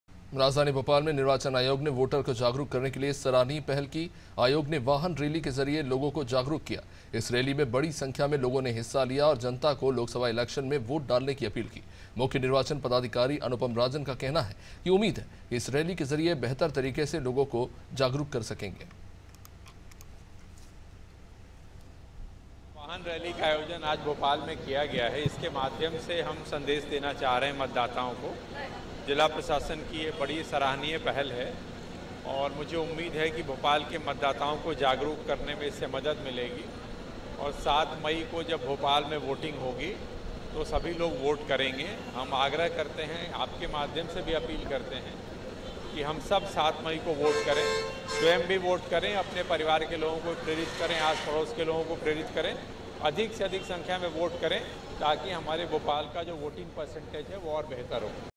BHOPAL : निर्वाचन आयोग की सराहनीय पहल, शहर में निकाली वाहन रैली || Anaadi Tv
AIRTEL - 379